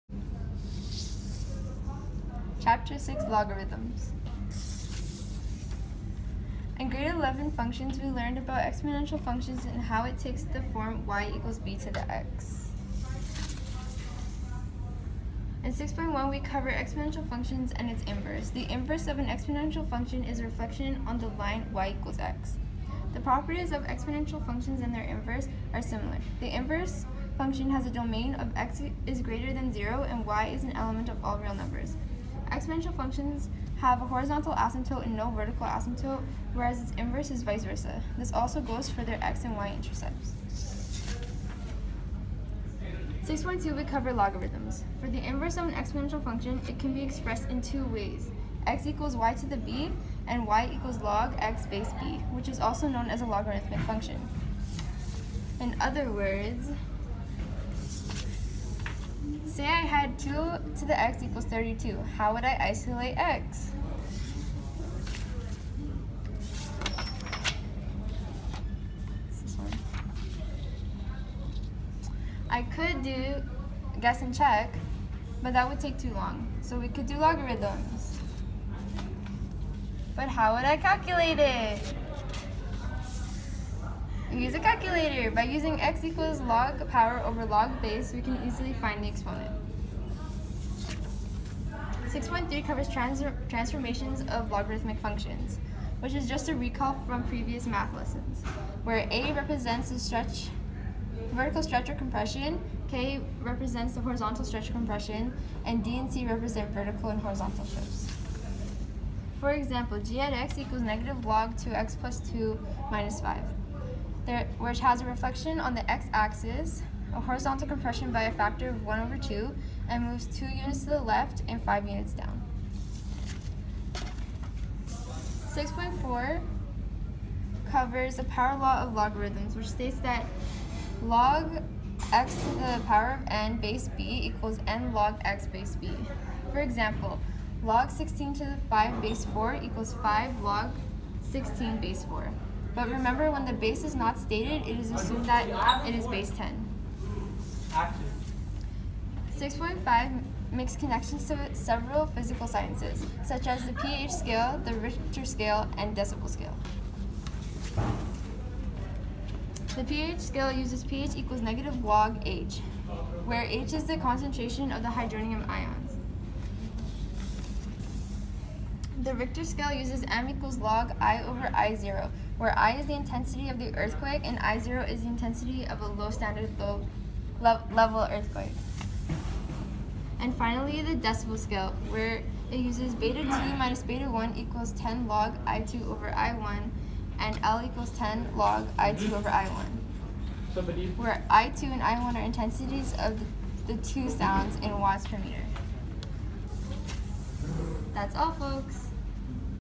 Chapter 6 Exponential And Logarithmic Functions Paper Slide Video By Adib, Morgan & Murtaza
If you need help learning math just watch this video and you’ll be blessed. Seriously !!! No joke !!!